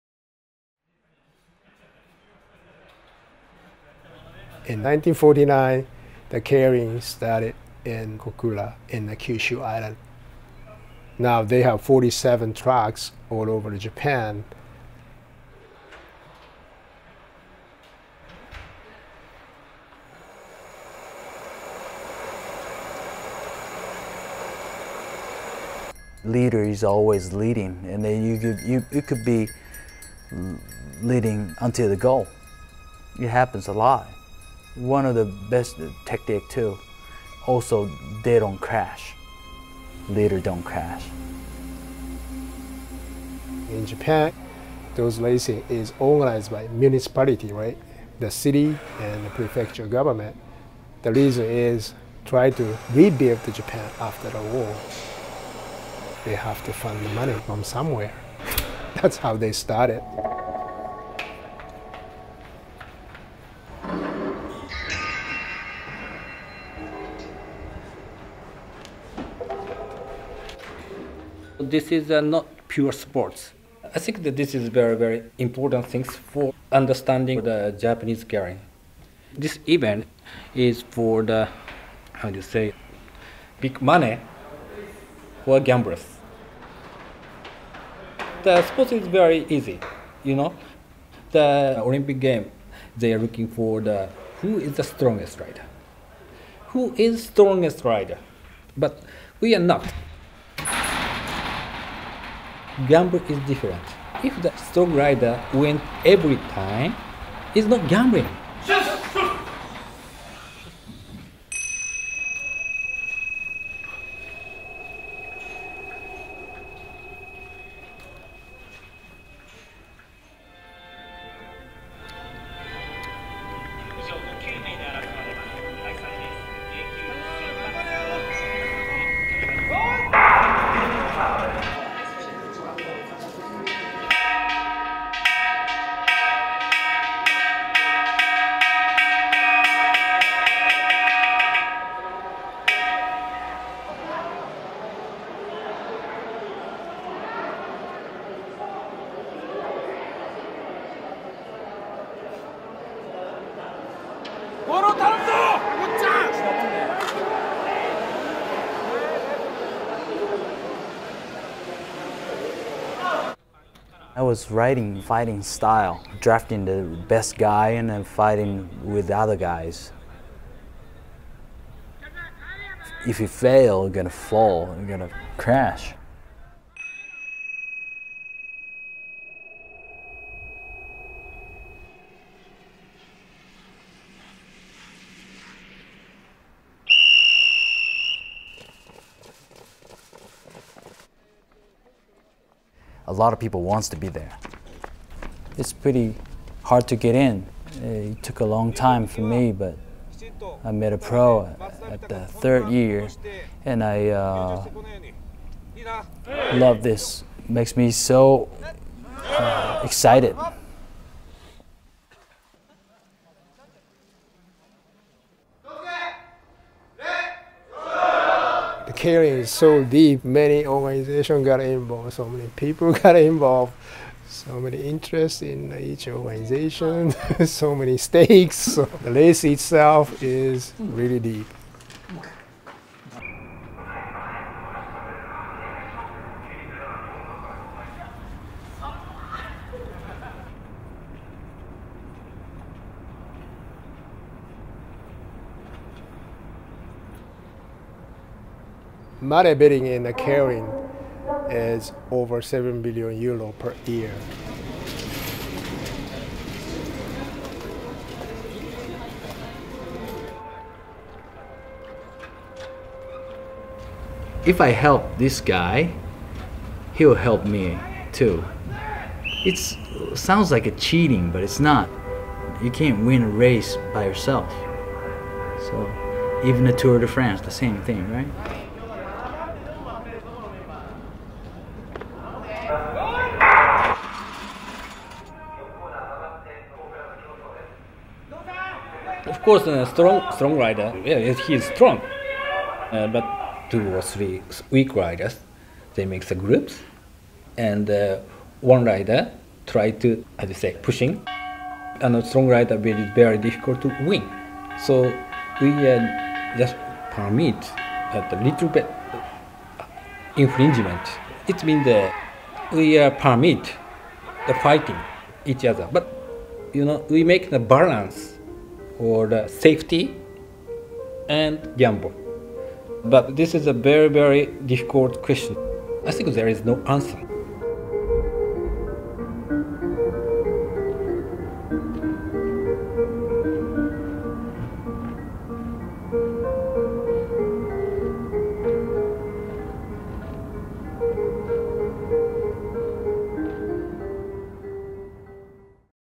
Keirin: Speed Racers - Watch Japan's Track Cycling Phenomenon
The fashion photographer gives NOWNESS the inside track on Japan's track cycling phenomenon, Keirin. For these greyhound-fast speed racers the stakes are high and the action is high-octane.